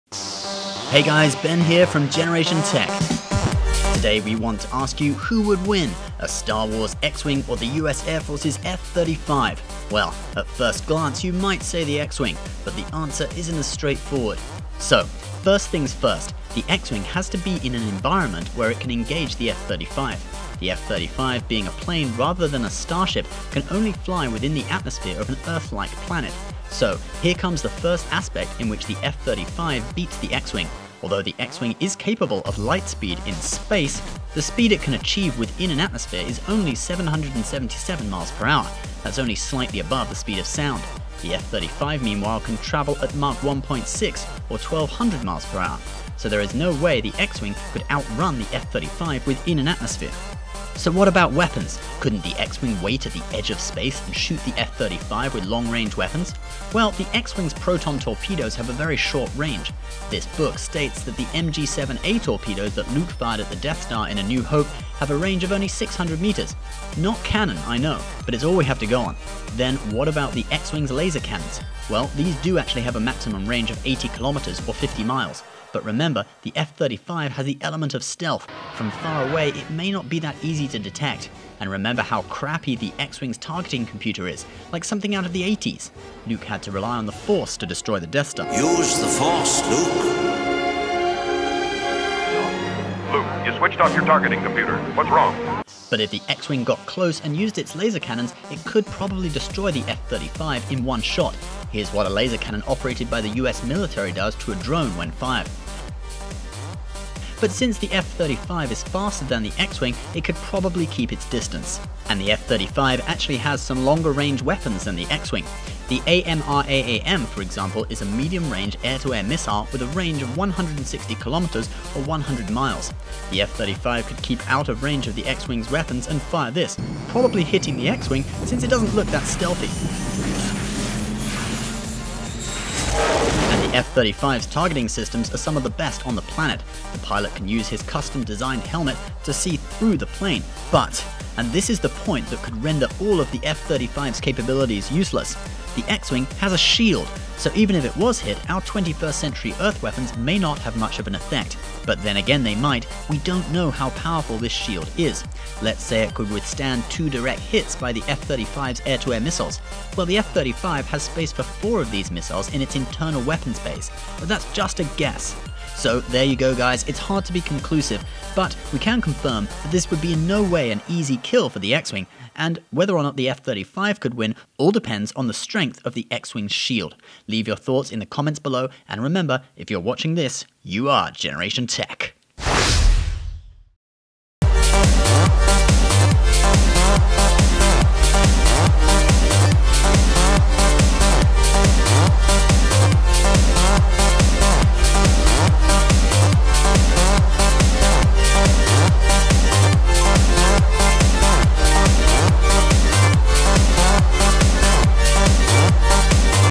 Which would Win: X-Wing or F-35? | Generation Tech
Check out our rundown of an X-wing VS F-35 fight, the outcome isn't quite what you would think.